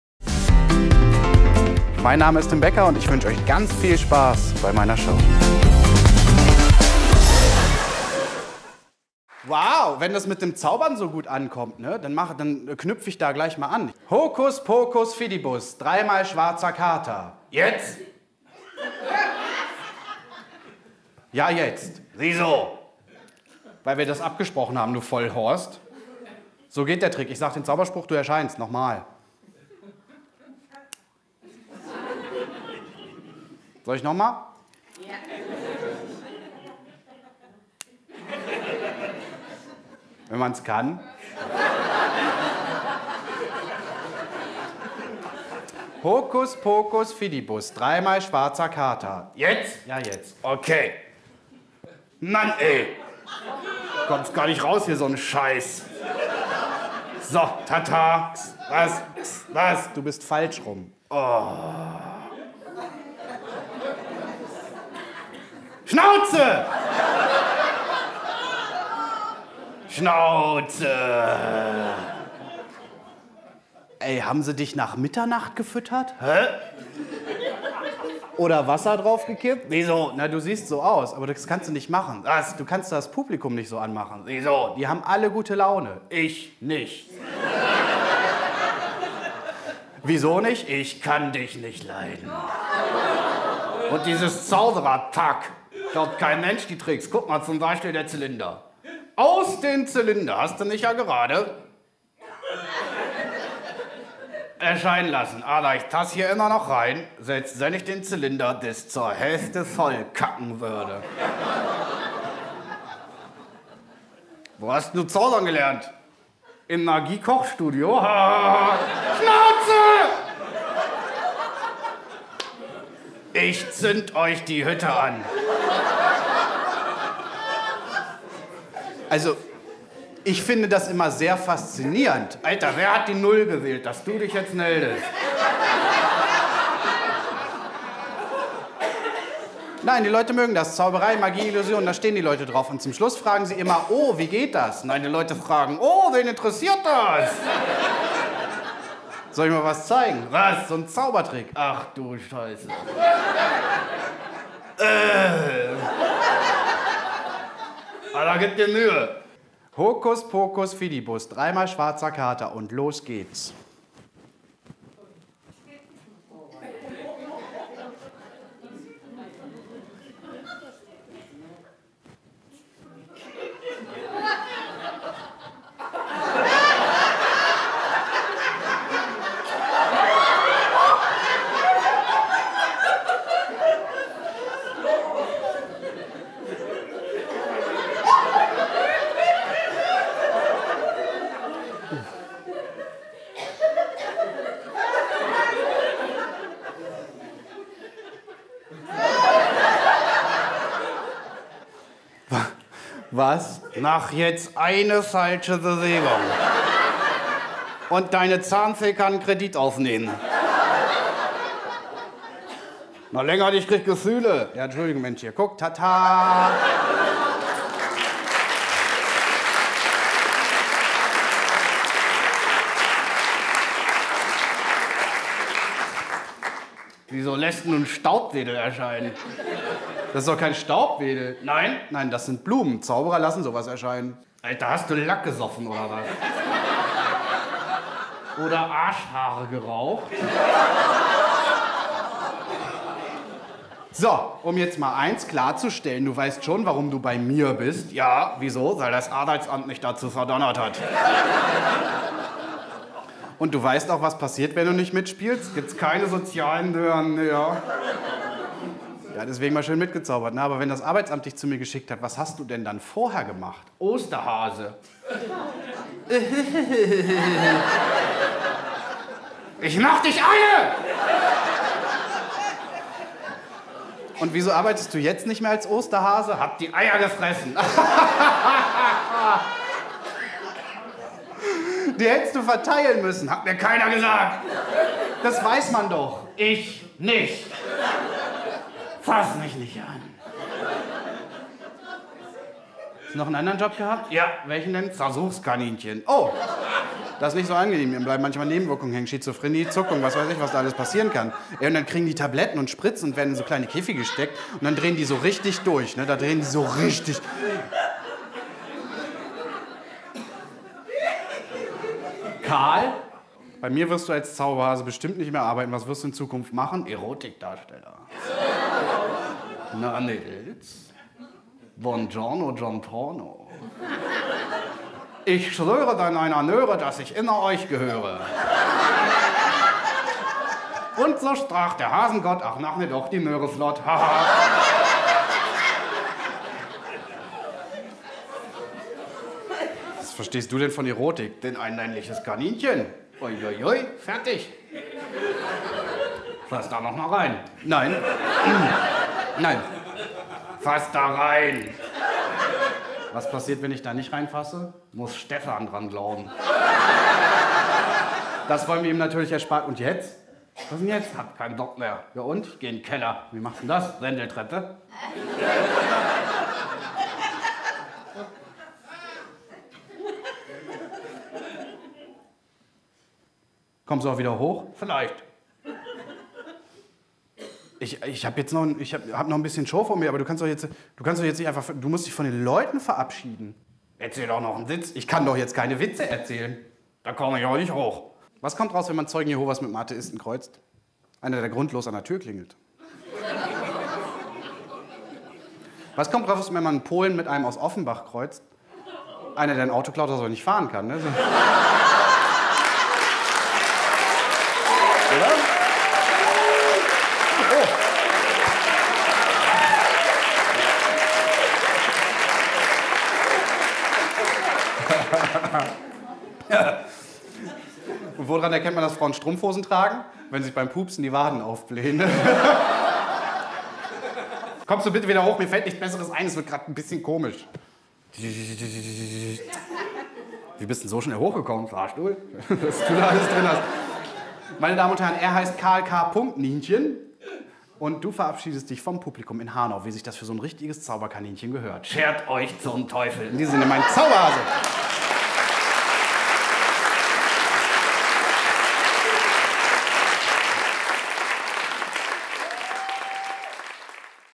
Bauchredner Tim Becker und sein Aggro- Zauberhase
Bauchredner Tim Becker präsentiert seinen Aggro-Zauberhasen Karl K. Ninchen, der so gar keinen Bock hat auf diesen Job.